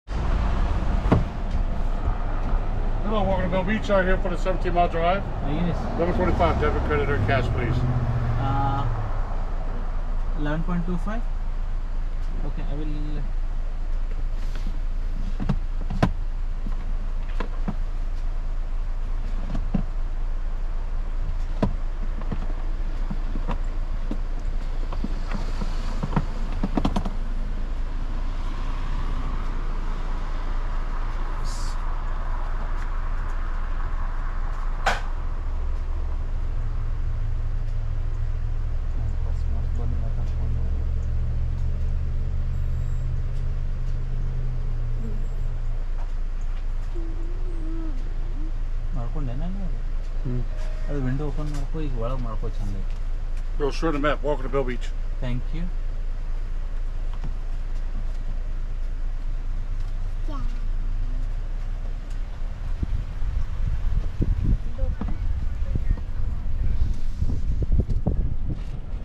17 mile drive Entrence
The famous 17 mile drive located in Montery of California located 90miles from Fremont city of Bay area